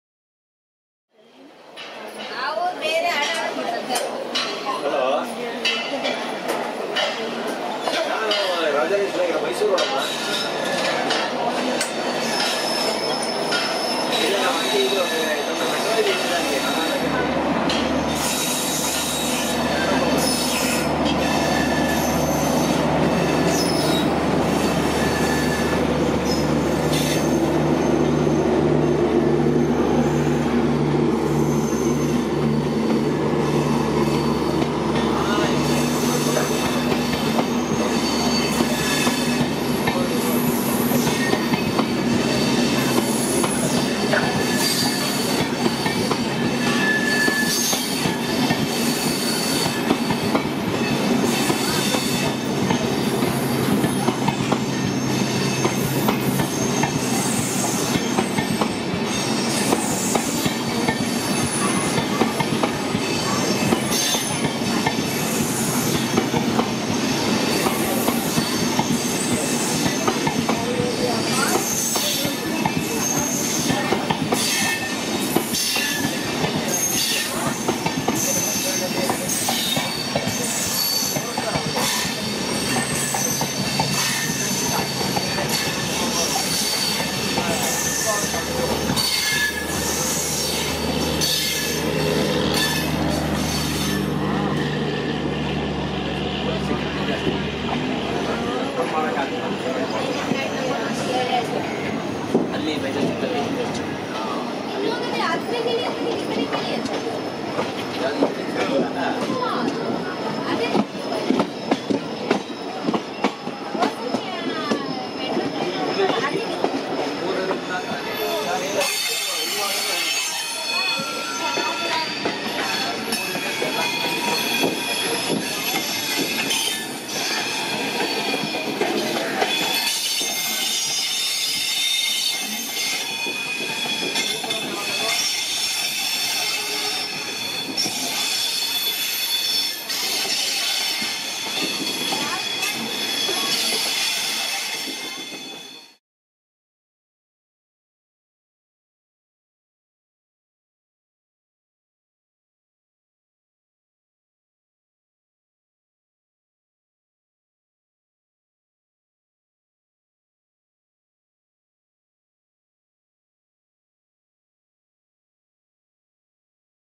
Screeching Train Wheels | Kochuveli AC Express departing Yesvantpur
The colorful mismatch LHB consists of the Kochuveli AC Express screeches on the Rails as it moves out of Yesvantpur hauled by a WAP4.The train shares its rake with  Yesvantpur - Delhi Sarai Rohilla AC Duronto Express.

Train : 22677 Yesvantpur - Kochuveli AC SF Express
Location : Yesvantpur
Locomotive : ERODE WAP4 22381.